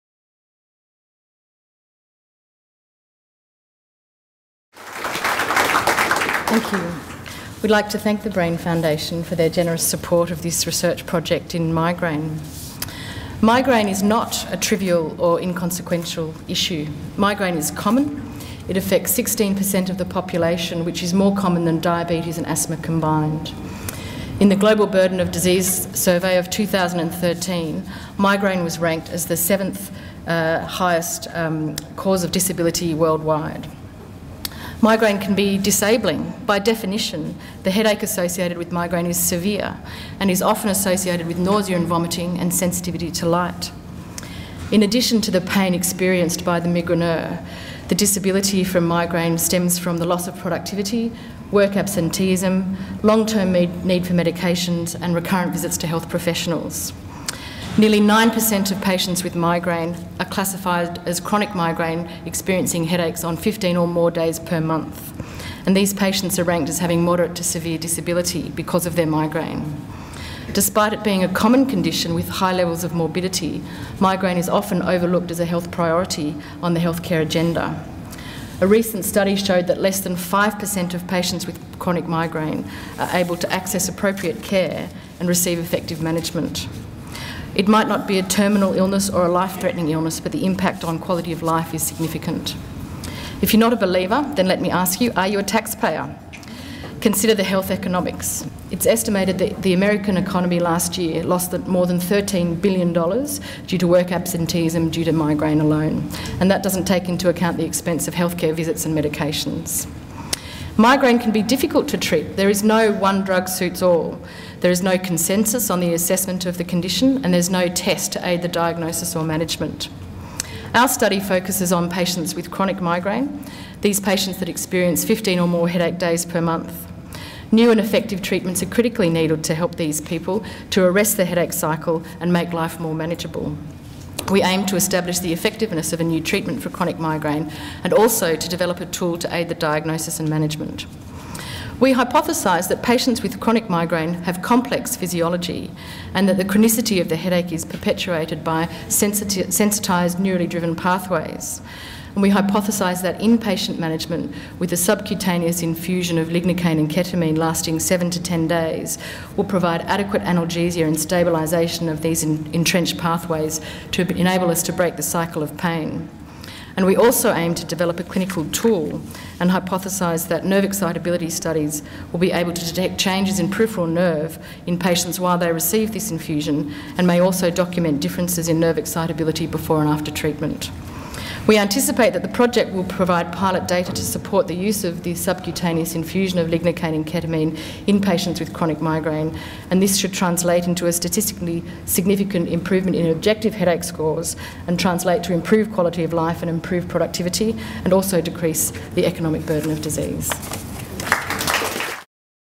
Brain Foundation 2016 - Dr Susan Tomlinson
Brain Foundation 2016 - Research Gift Awards - Acceptance speech by Dr Susan Tomlinson - Chronic Headache & Migraine - Making headway into migraine: assessment of subcutaneous lignocaine and ketamine use in the management of chronic (transformed) migraine and development of a biomarker of headache